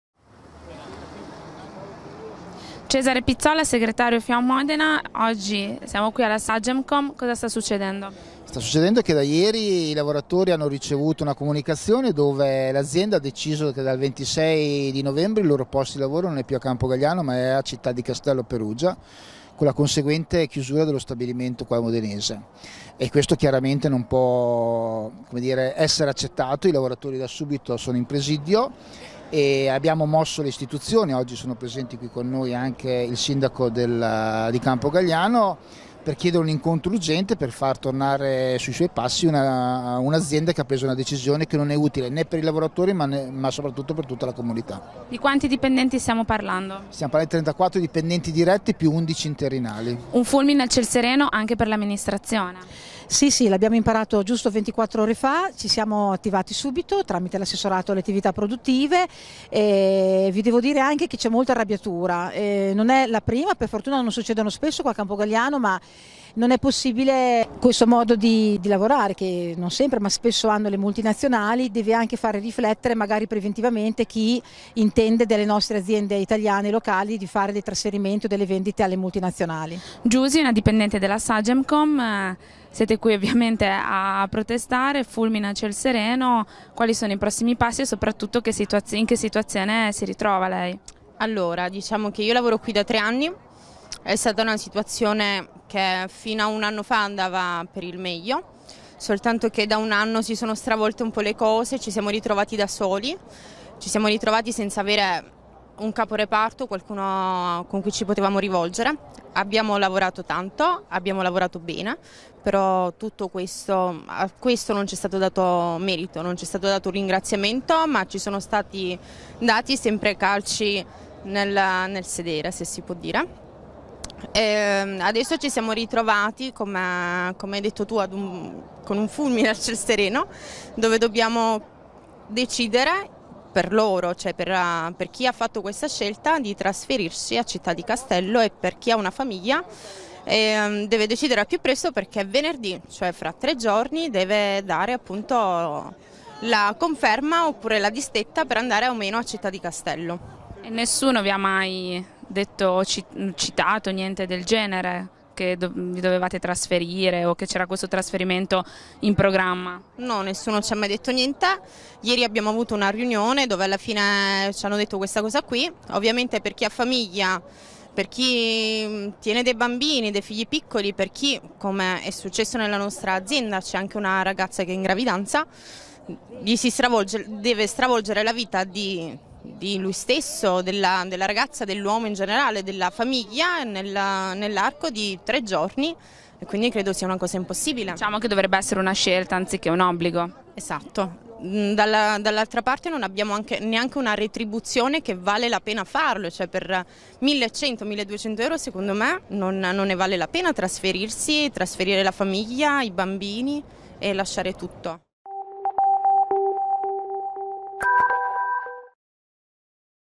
Campogalliano, i dipendenti Sagemcom in sciopero
La disperazione dei lavoratori della Sagemcom: "In tre giorni dobbiamo decidere se trasferirci". Video di Gino Esposito, intervista di Serena Fregni L'ARTICOLO